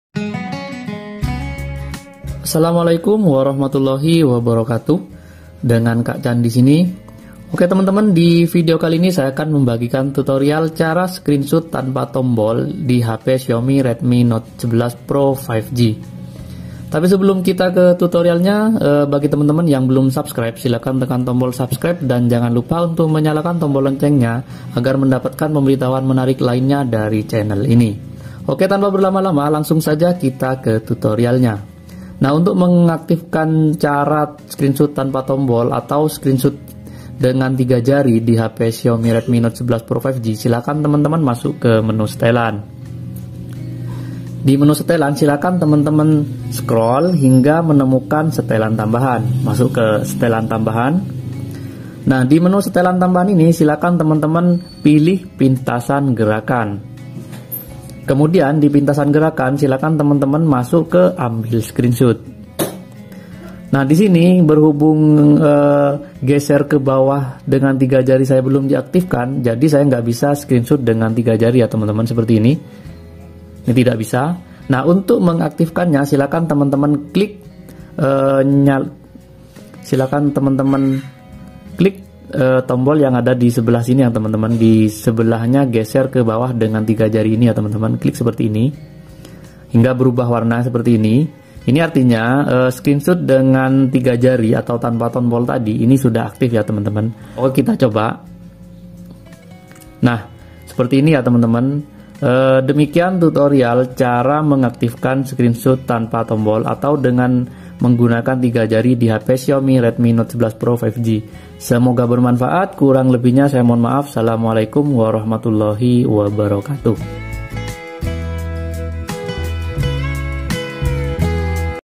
Cara Screenshot Tanpa Tombol Di Hp Redmi Note 11 Pro 5g | SS Xiaomi Tanpa Tombol Power
Berikut saya bagikan tutorial cara screenshot tanpa tombol di hp redmi note 11 pro 5g. Ss xiaomi tanpa tombol power.